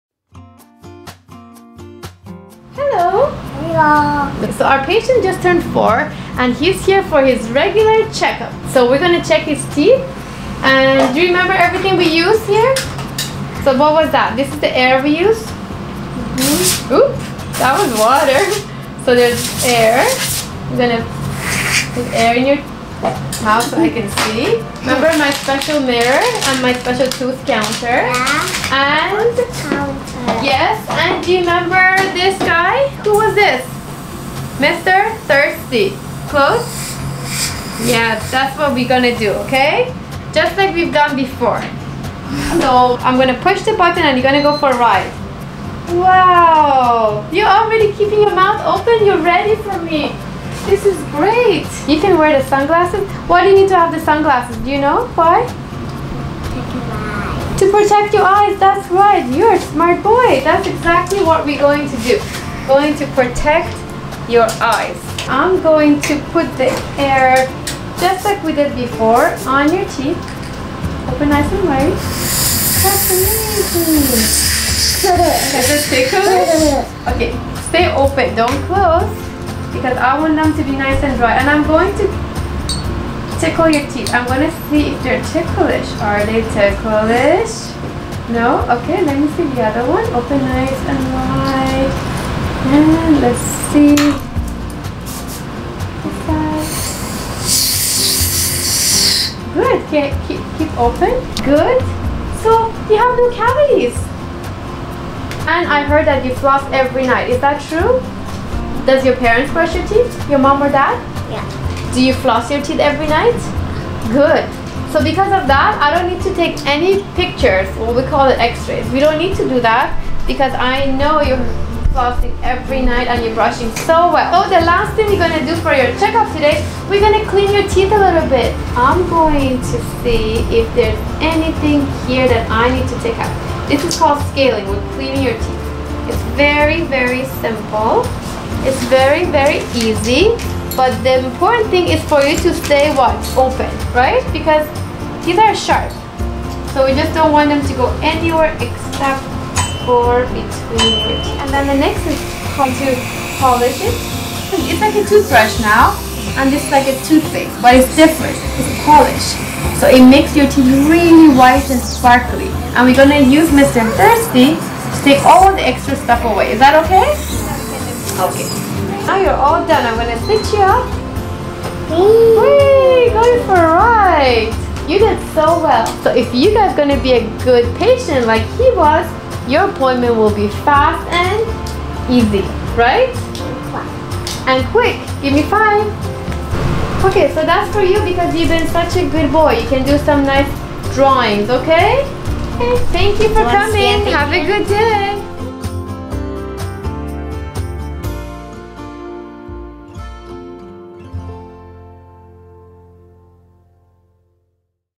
What to expect at your child’s dental checkup, cleaning and polish
Today our patient is here to demonstrate how fun and simple it is to have a dental exam, scaling and polish done.